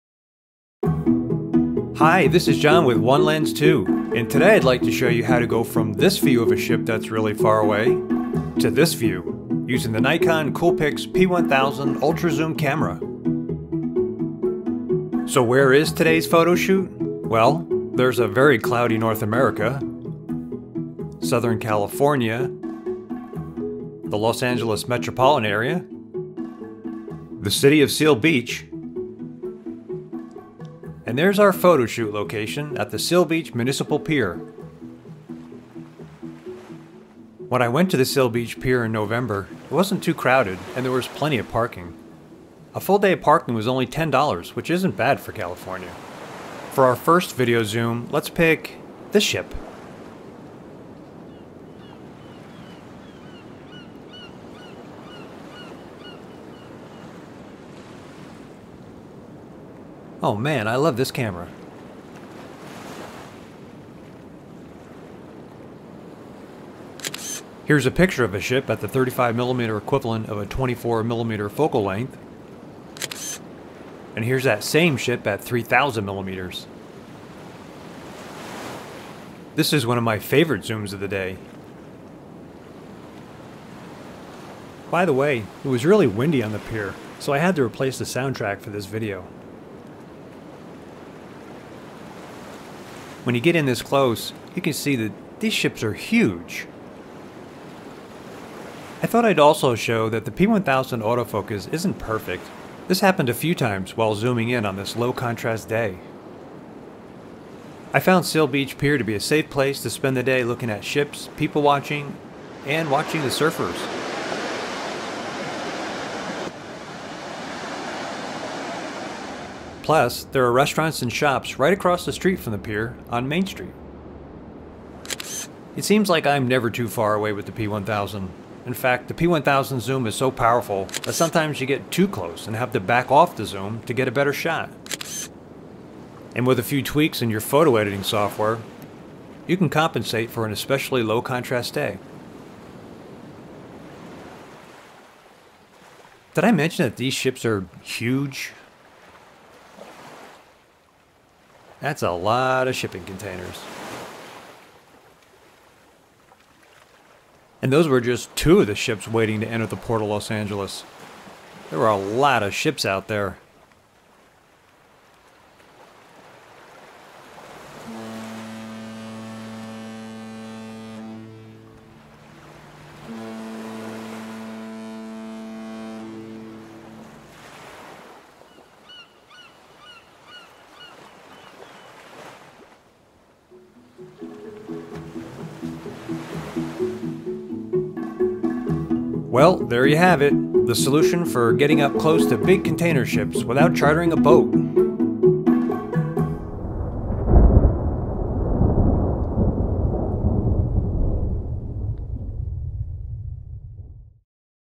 Super Zoom - Huge Ships Offshore - Port of Los Angeles - Nikon P1000
Due to the global supply chain delays, there are a lot of massive container ships anchored near the Port of Los Angeles in California.  There are some pretty big boats out there! You can hardly see many of them from the beach, so we’ll use the Nikon COOLPIX P1000 ultra-zoom camera to zoom in close from the Seal Beach Municipal Pier in Seal Beach, CA.

You can read my article about cargo ship watching at Seal Beach on my website here:

For more videos, pictures and articles on super zooming, check out 1lens2.com.